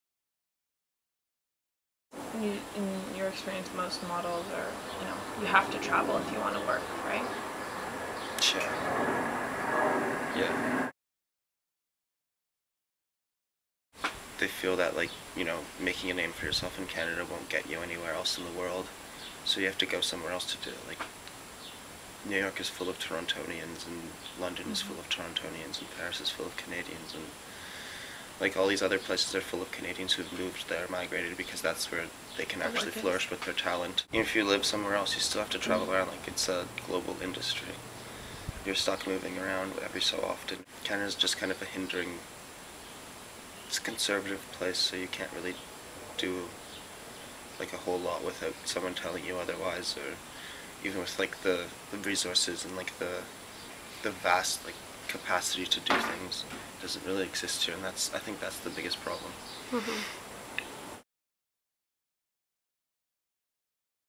John Cherkas: Canada's shortcomings
John Cherkas talks about the Canadian modelling industry.